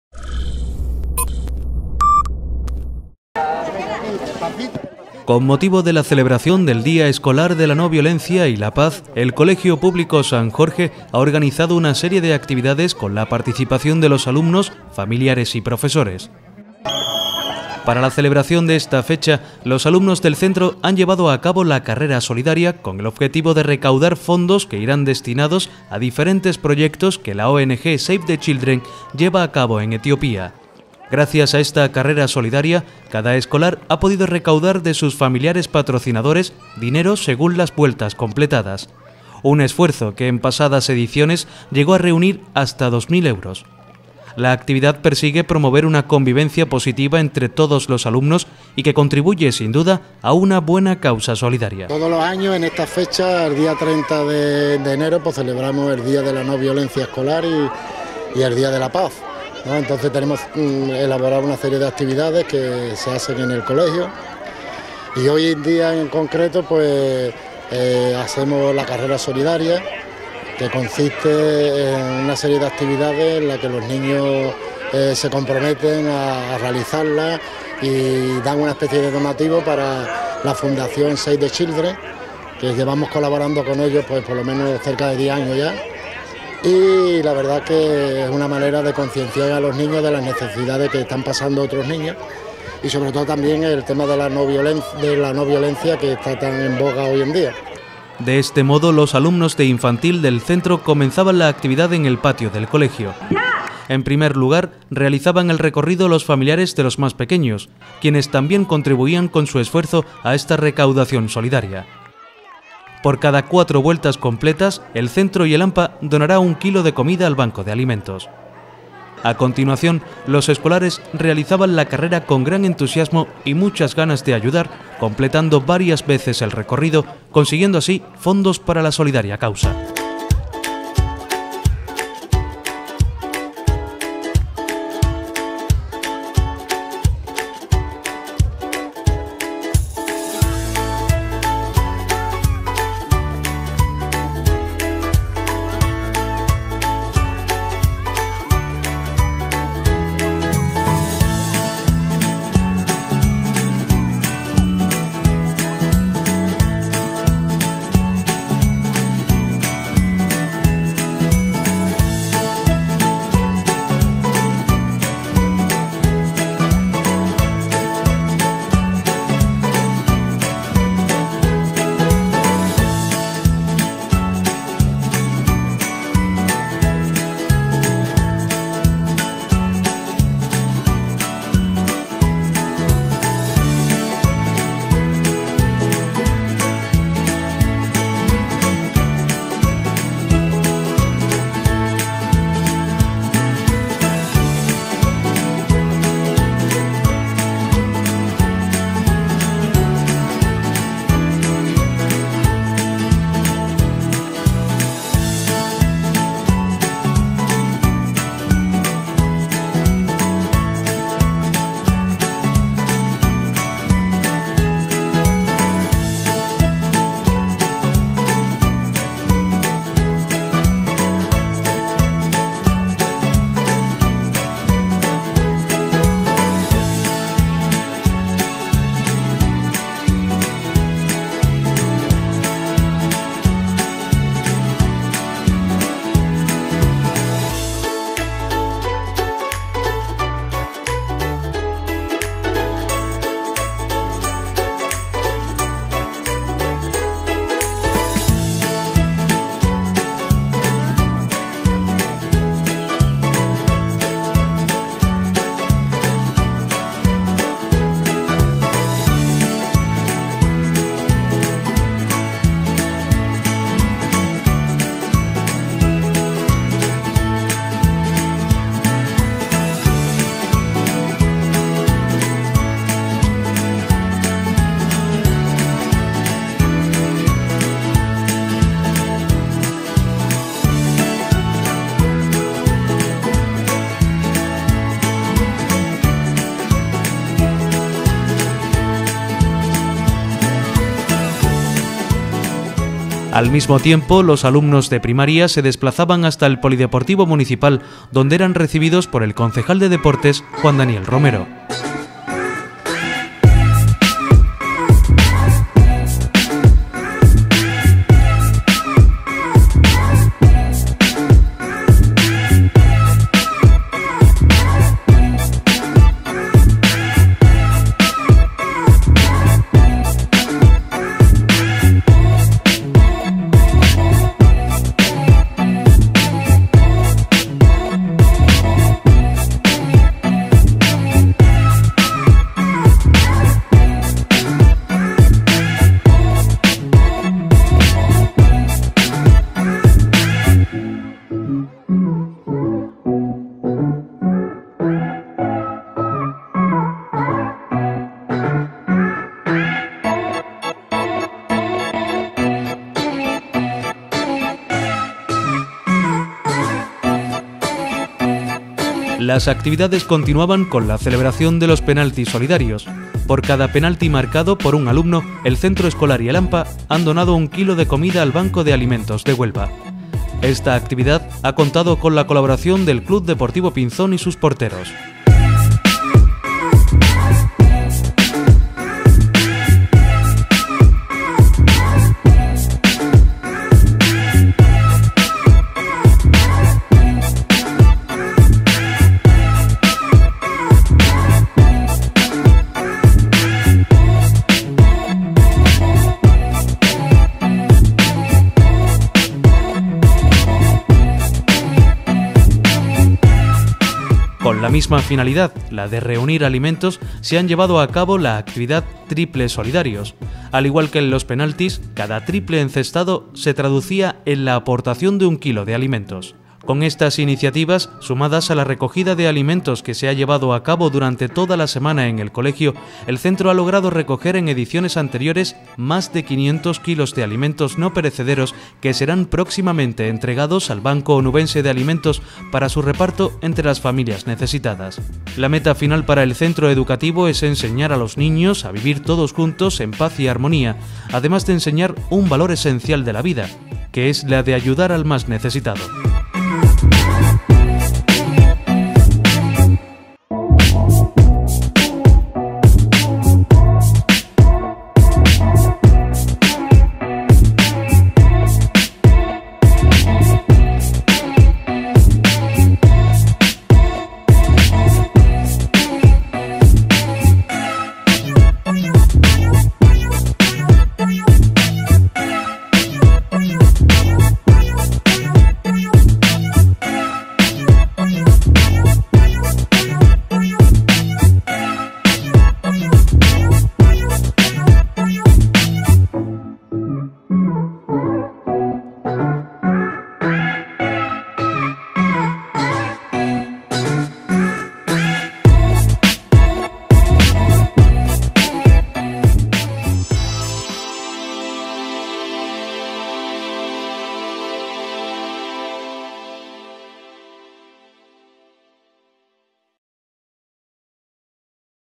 El colegio San Jorge celebra el “Día de la Paz”
Con motivo de la celebración del “Día escolar de la no violencia y la paz”, el colegio público San Jorge ha organizado una serie de actividades con la participación de los alumnos, familiares y profesores.